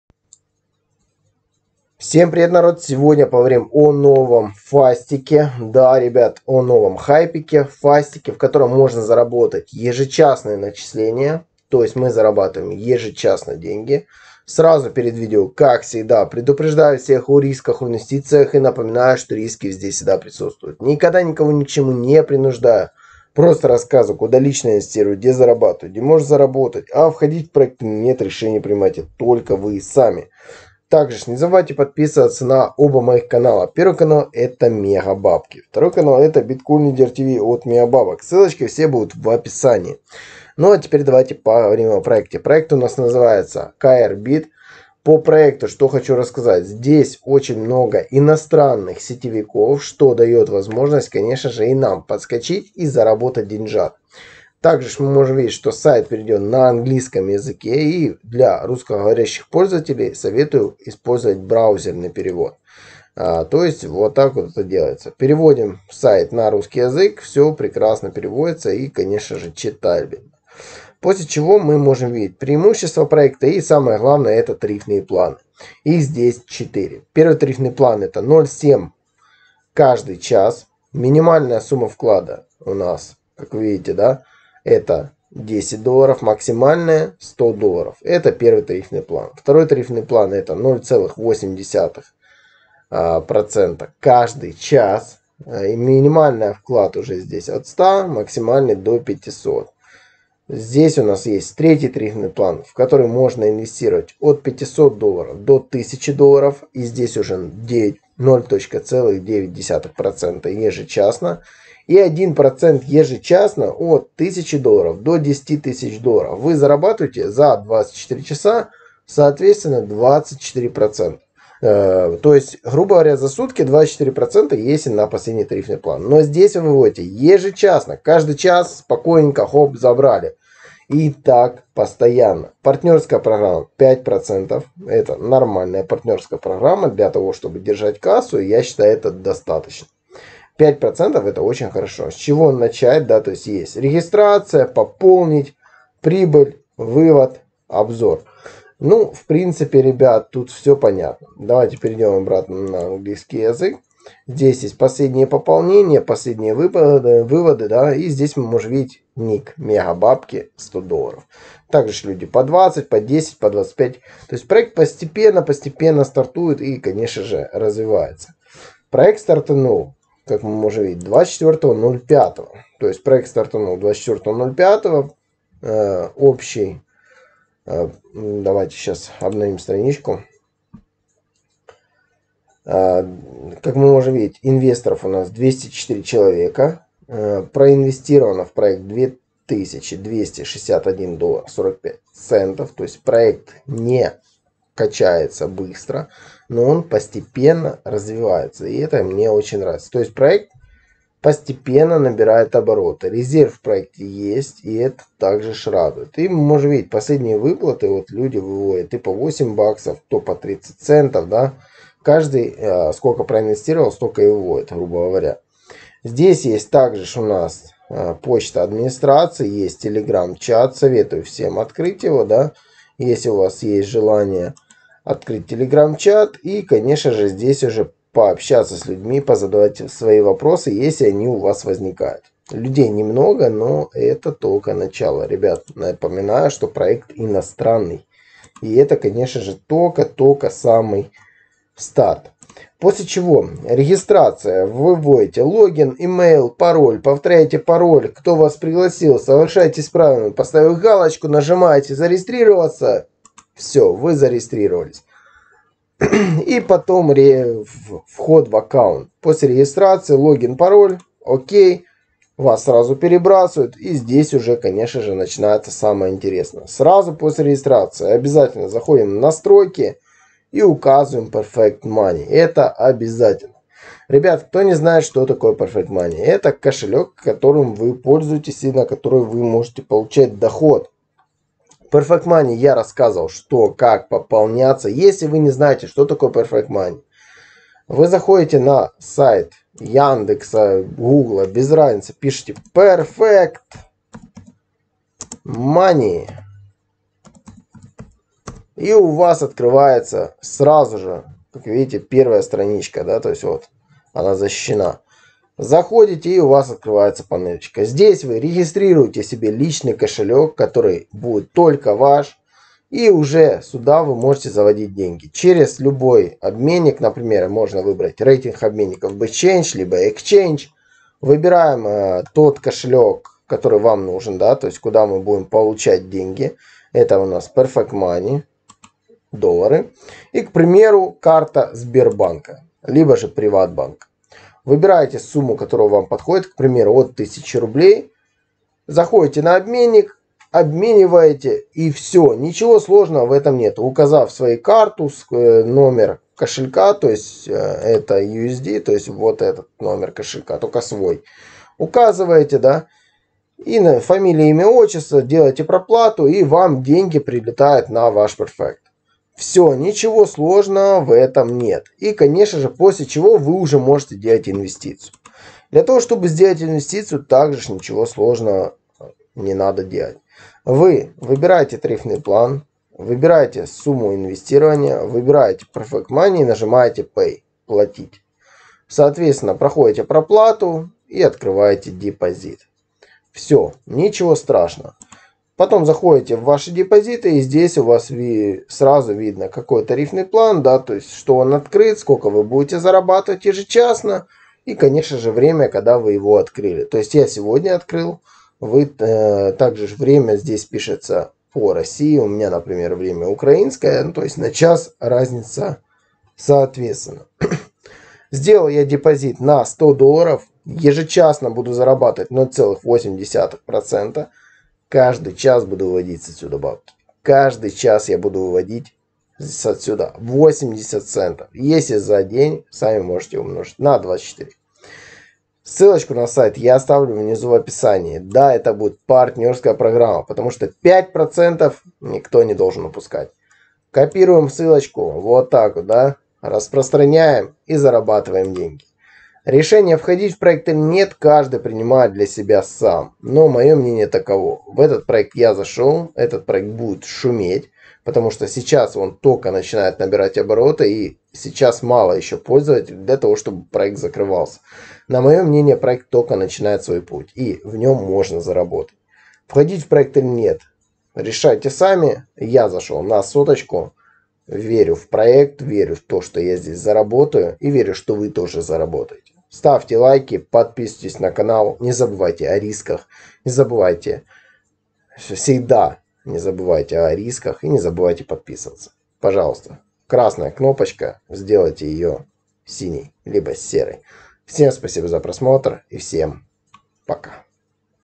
cr-bitt.pro новый хайп зарабатывать каждый час реально
проект скам не  платит